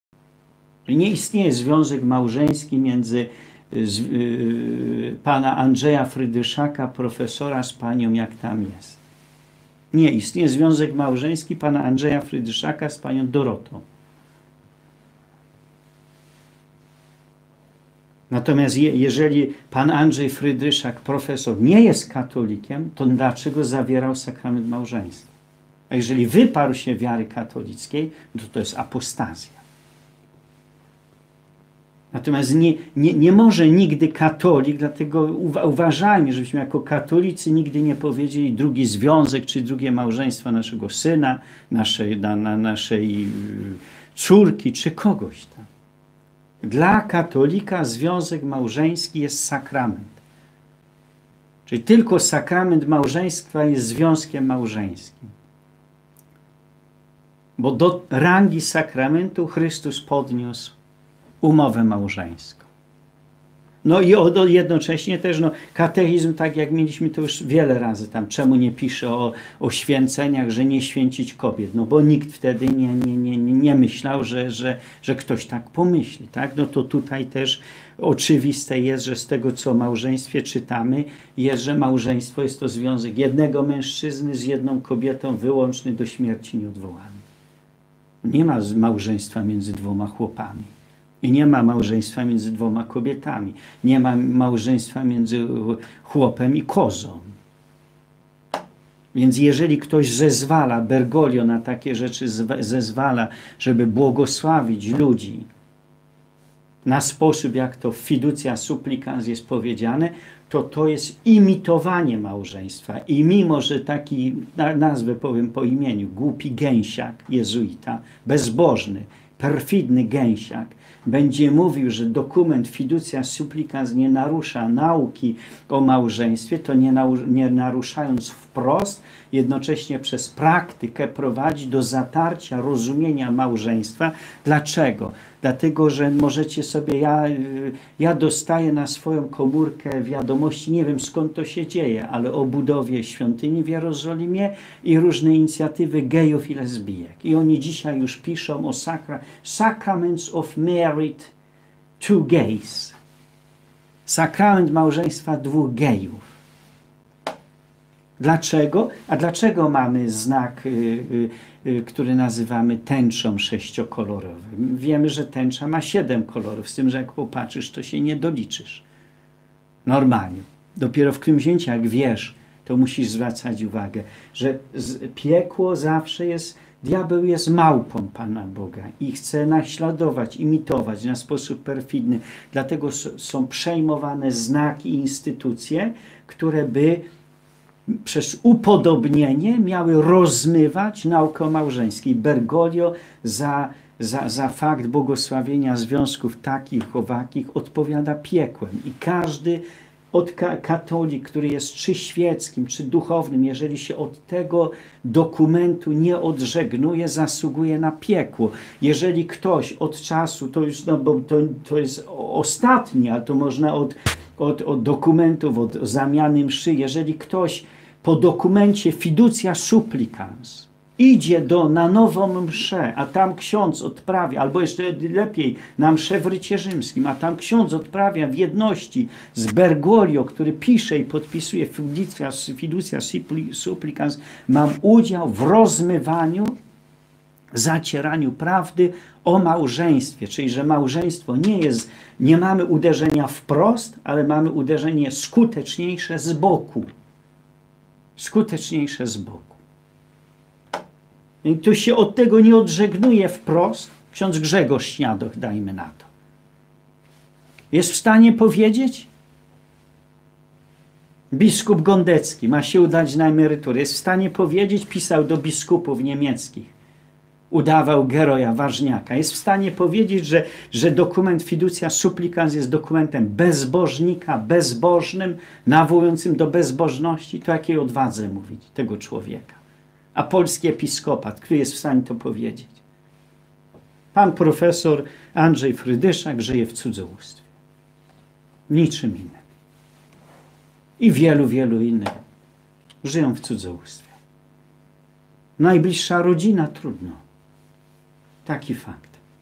MOJA RODZINA PROF. FRYDRYSZAK ŻYJE W CUDZOŁÓSTWIE WOŹNICKI W CZASIE KATECHEZY O MAŁŻEŃSTWIE 25.03 1
Michał Woźnicki...Były salezjanin i ksiądz Kościoła Katolickiego, wyrzucony z klasztoru Salezjanów w Poznaniu wciąż nie przestaje zadziwiać i szokować. .